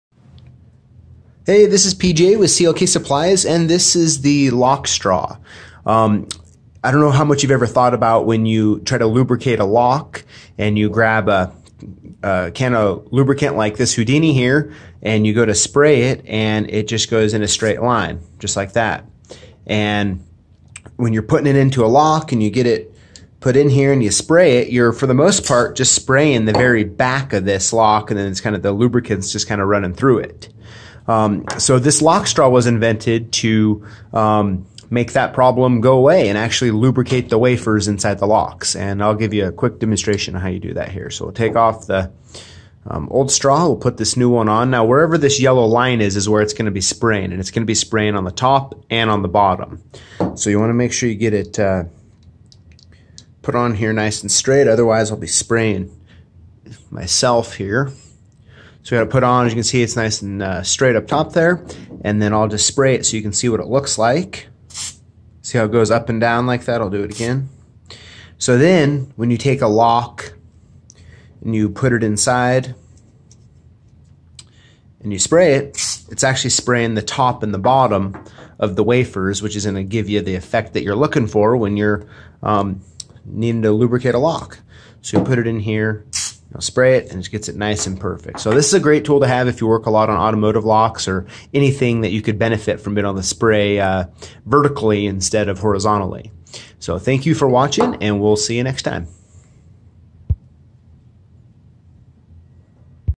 How to lubricate a Lock the right way! The Lock Straw
The Lock Straw! Have you ever wondered why after you spray lubricant into a sticky ignition it's still sticky? Well it's because your spraying the back of the lock! You need/want to spray vertically to lubricate the wafers! That is why the lock straw is a must have for ANYONE who works with automotive locks.